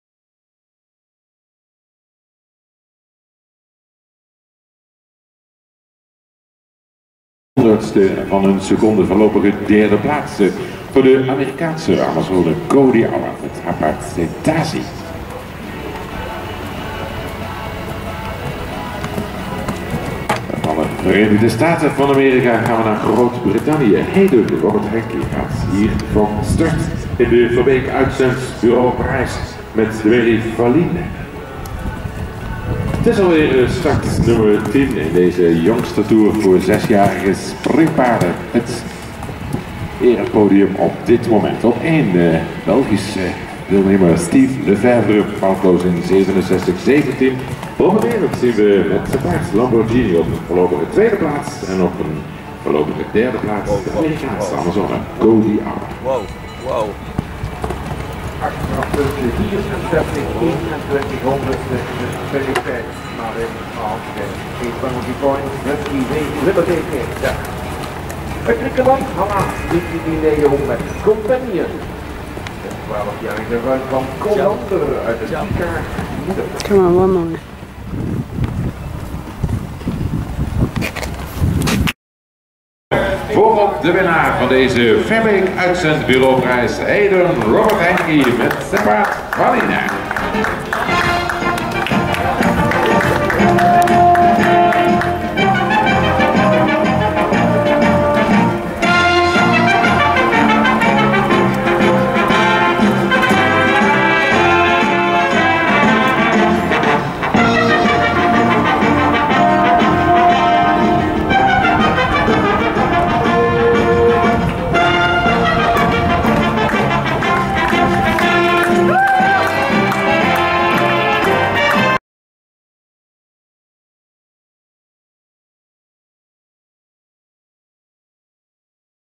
Faline (Quality Time) 1st place CSI** SOLD*
6 year old mare winning 6 year old class CSI** Kronenberg 2016.